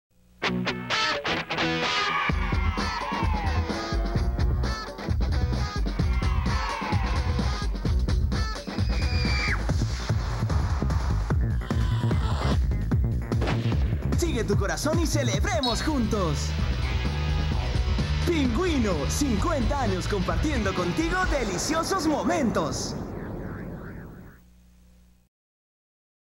Helados Pingüino - 50 años
Publicidad para televisión de Helados Pingüino con motivo de sus 50 años de vida en el Ecuador